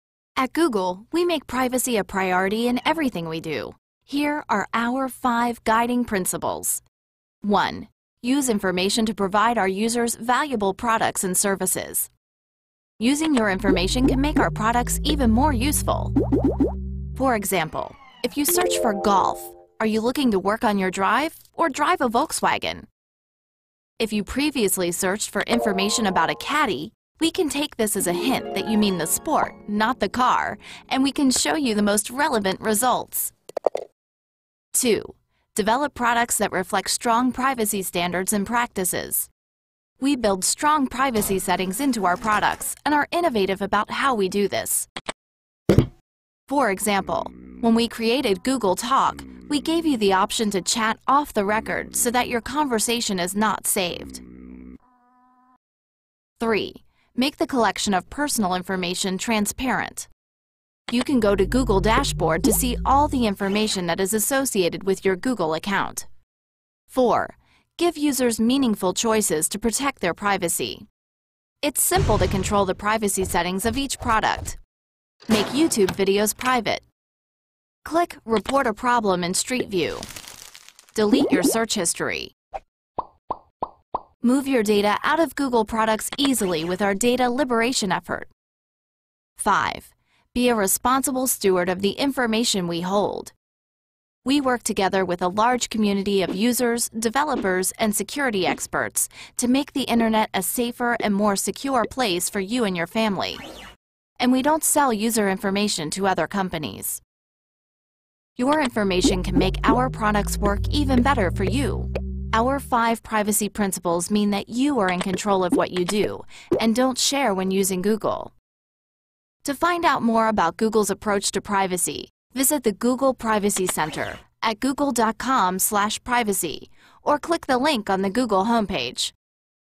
Good to Know
Google takes account security very seriously, and we regularly provide our users with tips to help protect their Google account and their computer. Here are 5 easy ways to help you stay safe and secure as you browse the web.